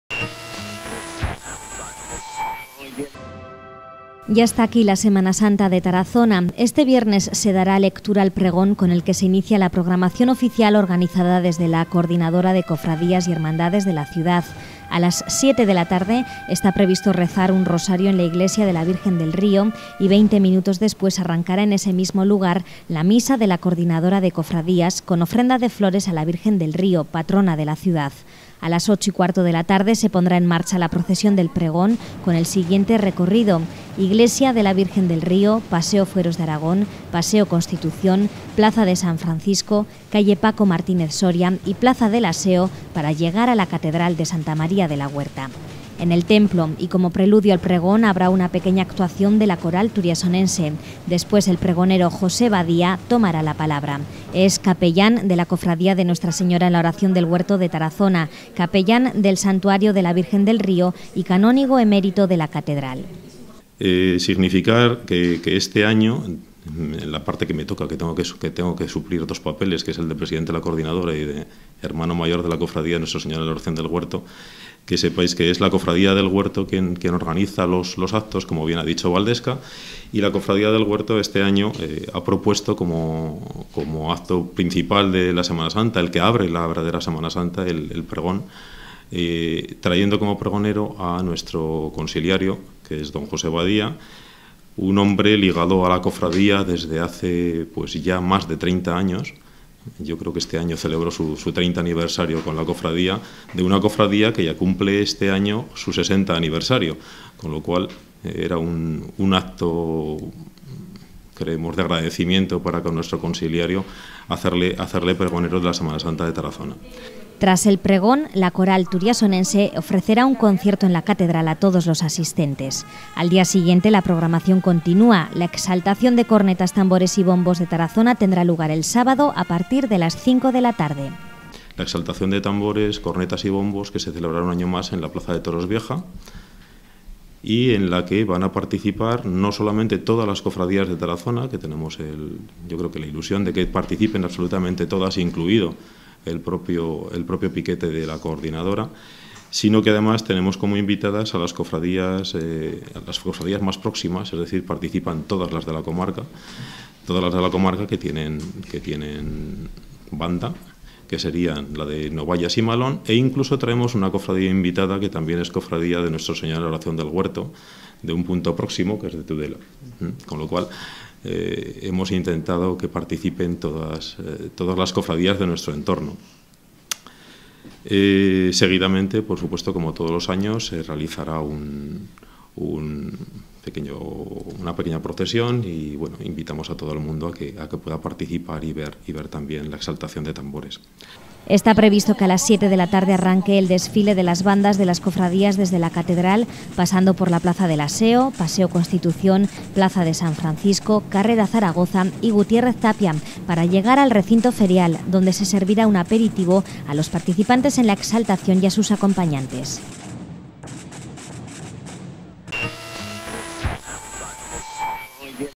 El pregón da el pistoletazo de salida a la Semana Santa de Tarazona
Tendrá lugar el viernes día 11 en la catedral de Santa María de la Huerta. Al día siguiente, la plaza de Toros Vieja acogerá la exaltación de cornetas, tambores y bombos.